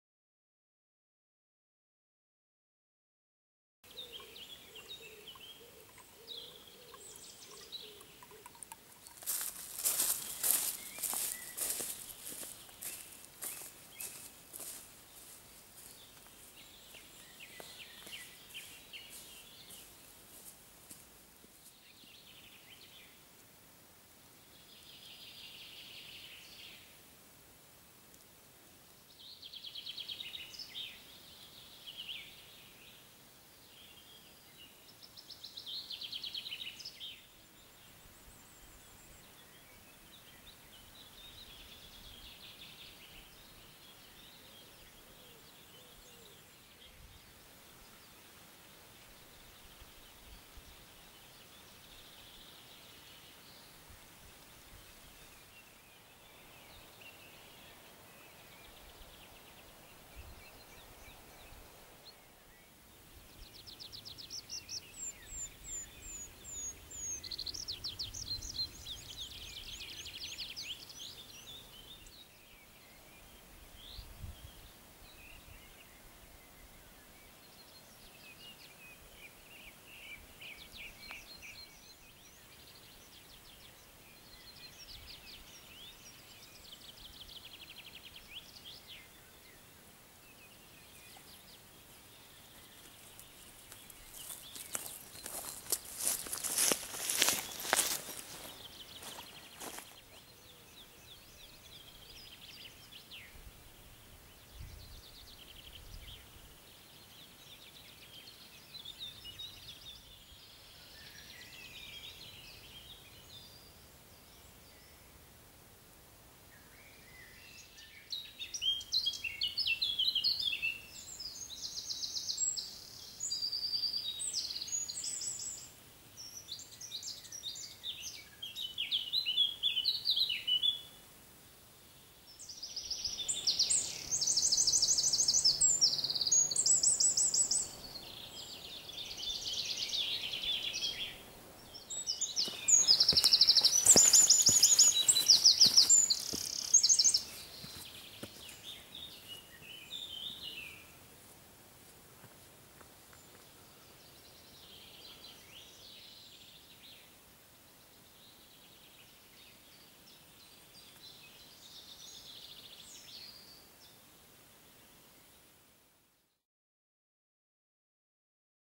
Randonnée dans les Vosges (Grand Ventron - Vintergés)
Randonnée au Grand Ventron, secteur de la Chaume des Vintergés. Lieu de calme, de pleine nature et de superbes forêts.